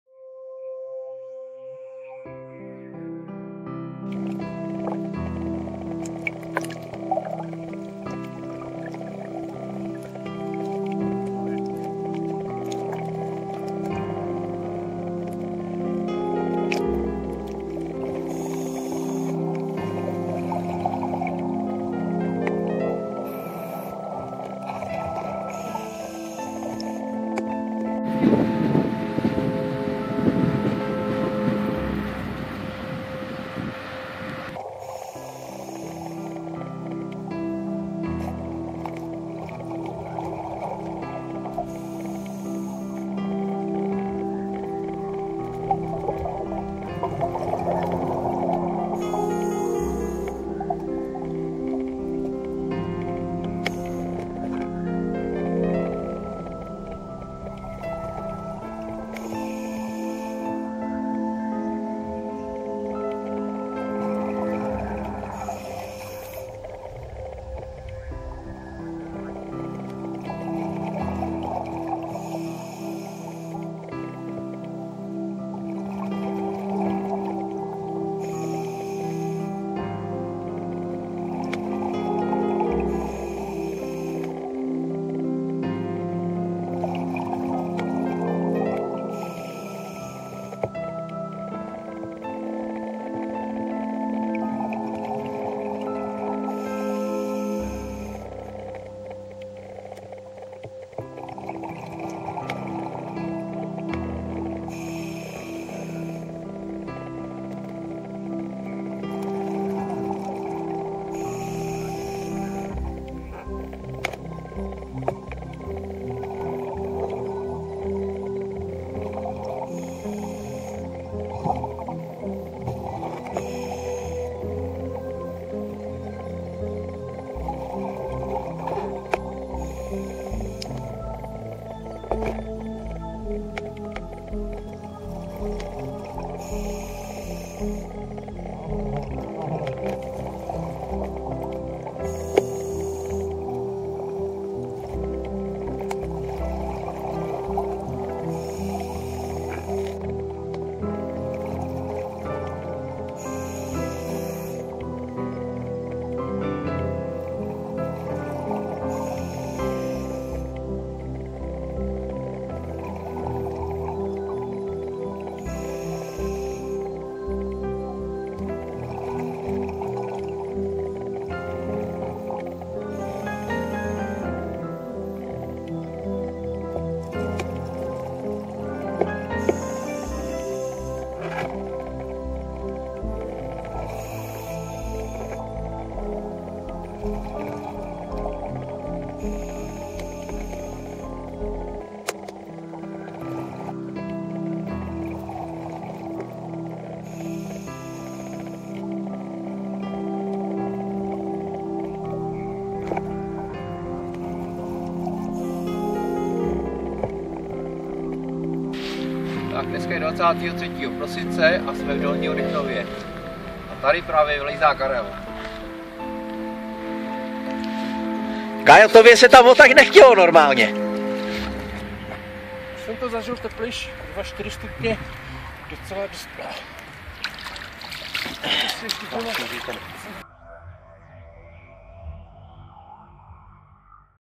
potápění v Dolním Rychnově 23.12.2013
Na Prosinec počasí spíš jako na jaře. Telota vzduchu 7st. teplota vody 4st.
Viditelnost do 7m se pohybuje mezi 3-5m nádhera.